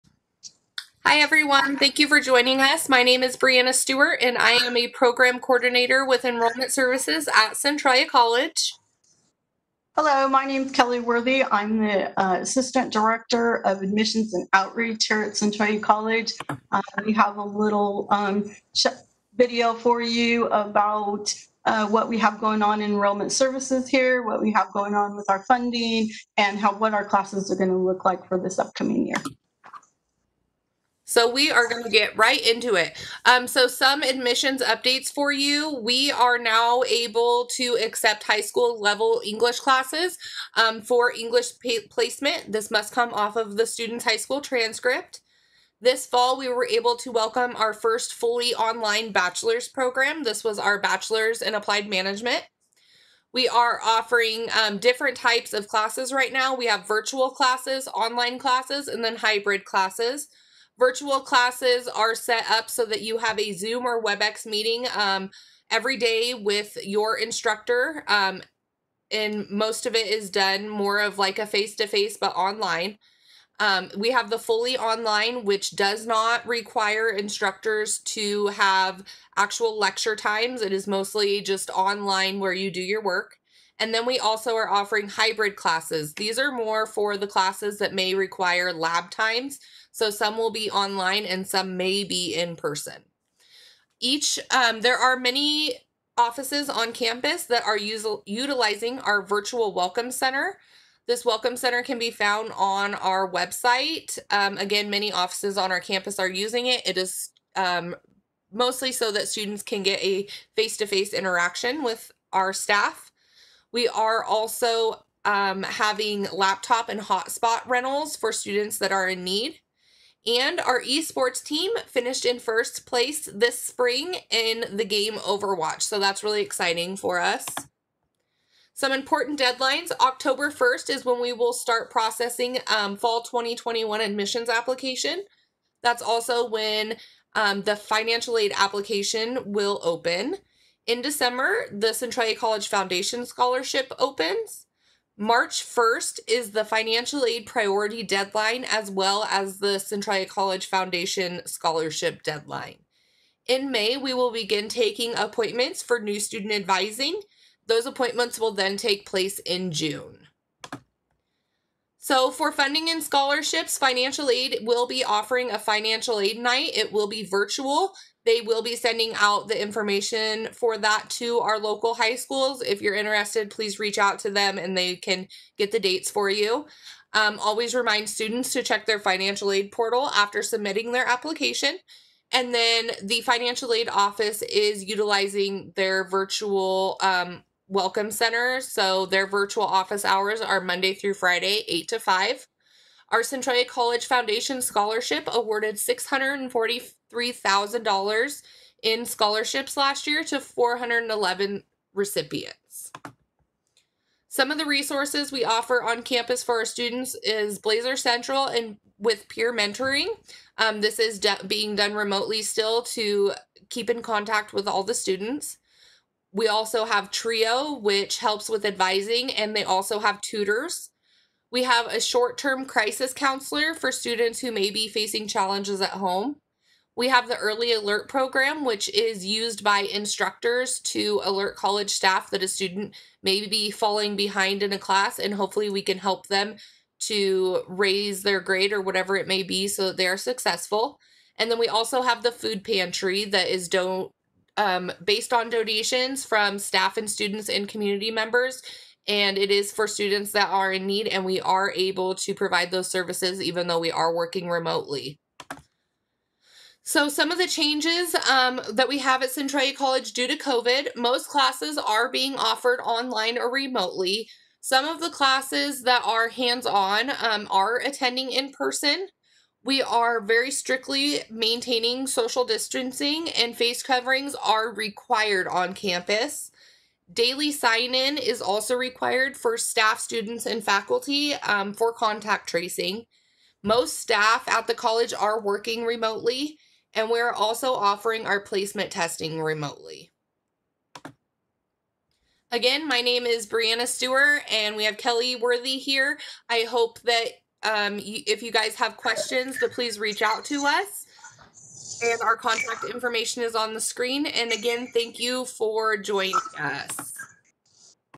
Centralia College 2020
Centralia College Video for the Fall 2020 Counselor Workshop hosted by The Washington Council